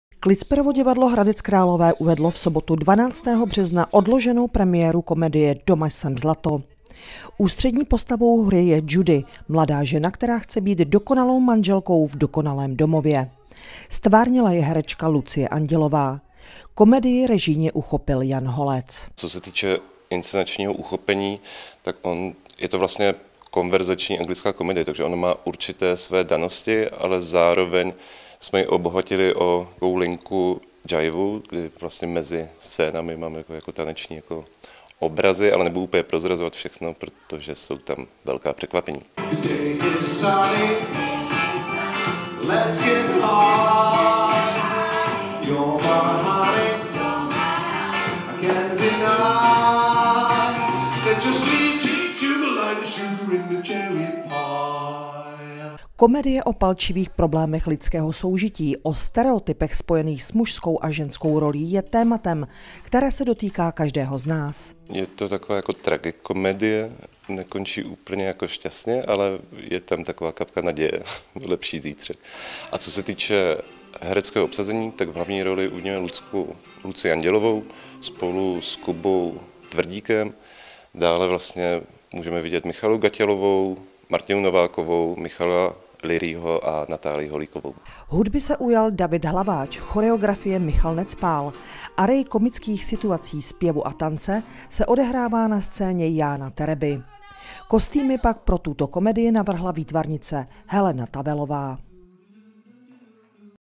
Klicperovo divadlo uvedlo premiéru hry Doma, jsem zlato!
Klicperovo divadlo Hradec Králové uvedlo v sobotu 12.3. odloženou premiéru komedie britské dramatičky Laury Wade – Doma, jsem zlato! v režii Jana Holce.